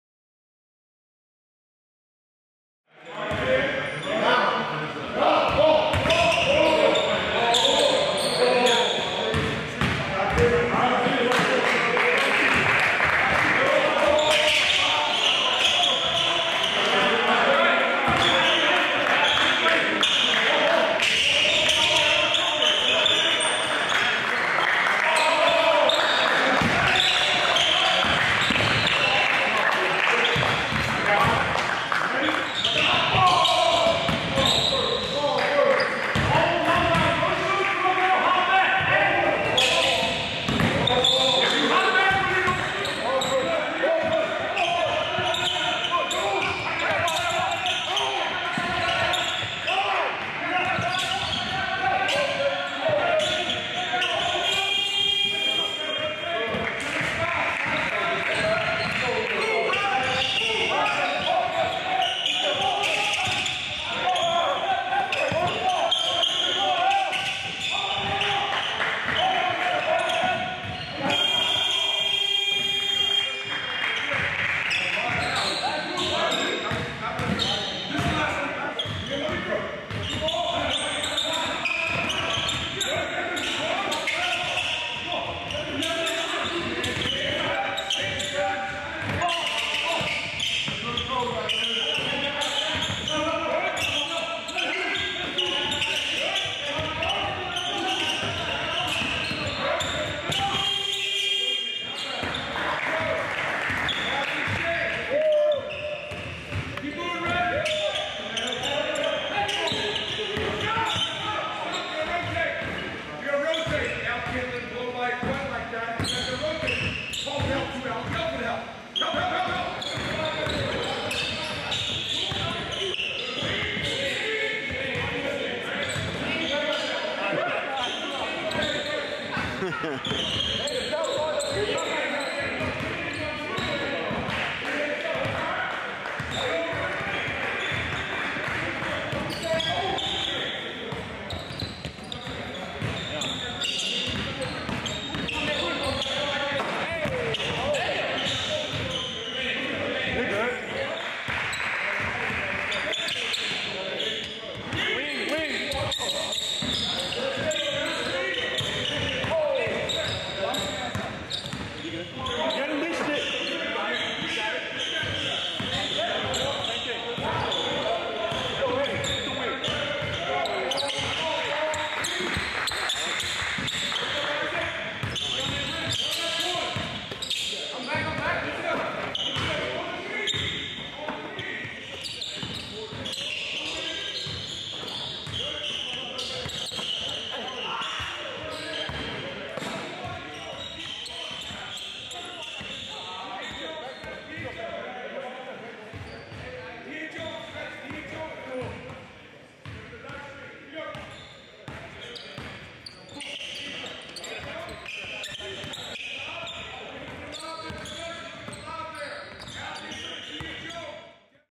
Auburn Tigers Basketball: Second Day of Practice
This is footage from Auburn's second day of basketball practice ahead of the 2019-20 season.